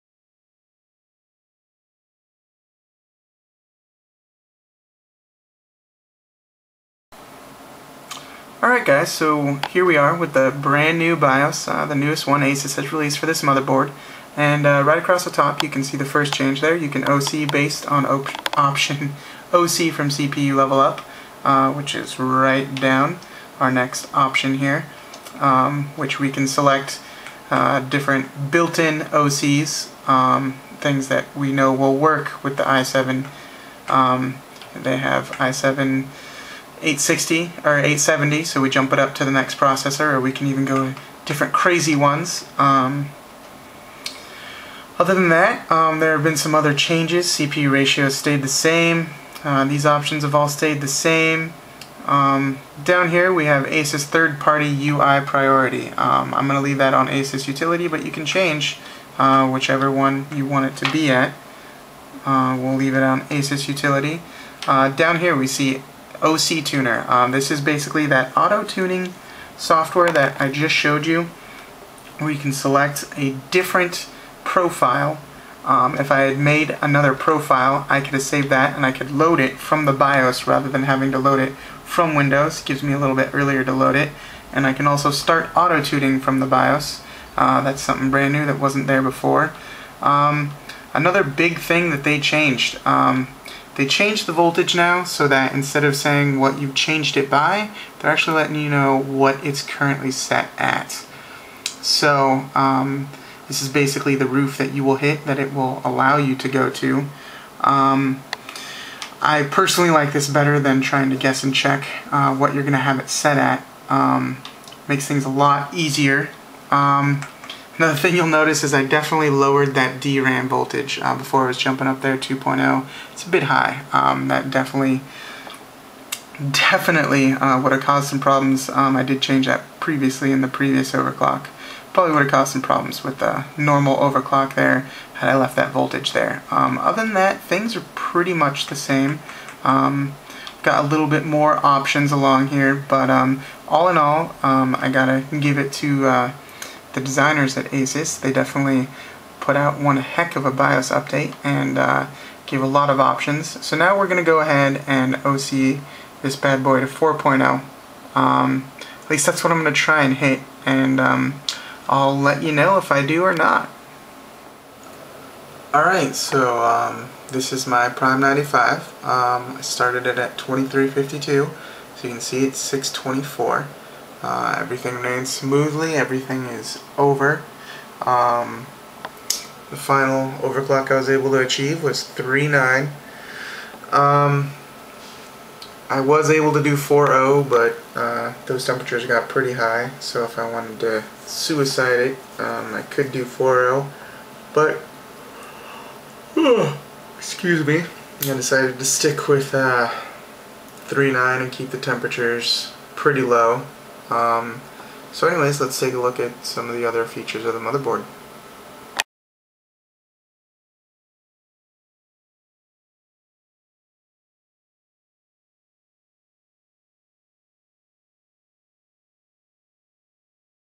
Overclocking via updated BIOS
This is the video showing of ASUS' new BIOS, version 1207, for the P7P55D premium motherboard. We were able to get the processor up to 3.9GHz using the new BIOS. I would have gotten 4.0, but the temps got too high for my liking (around 95c). Thanks ASUS for the parts and the opportunity to have so much fun!